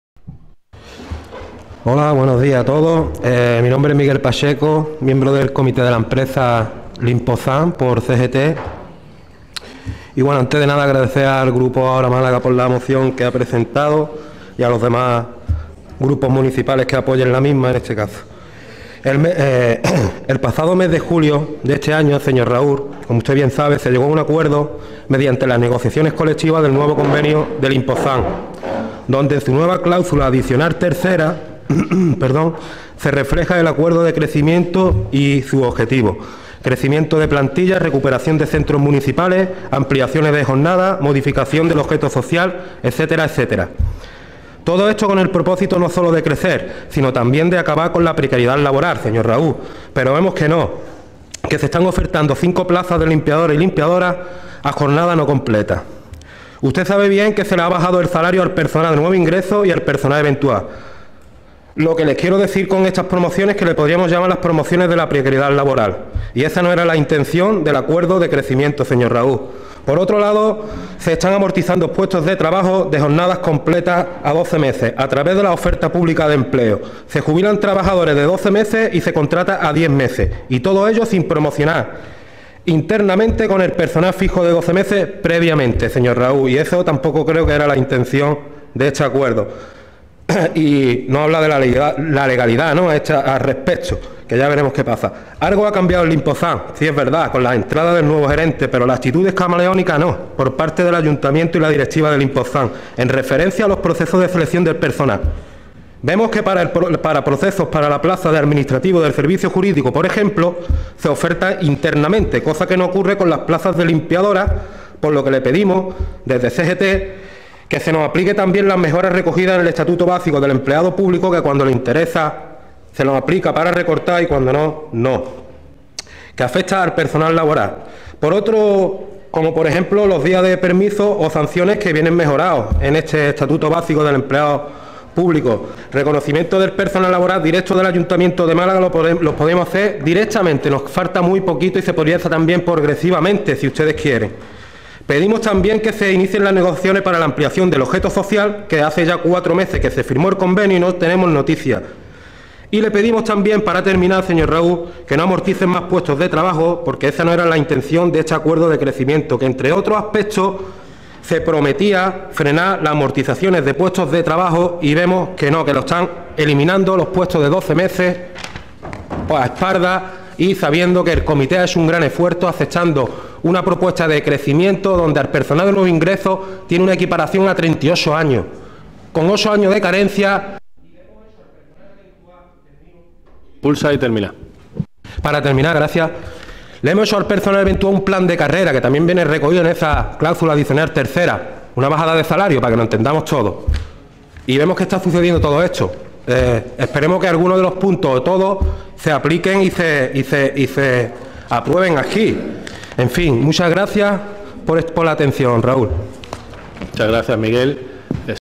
Intervención de Miguel Pacheco (CGT) sobre Limposam
Intervención de Miguel Pacheco (CGT) sobre la moción para conseguir que la plantilla de 365 trabajadoras de Limposam adquieran el status de personal laboral  municipal.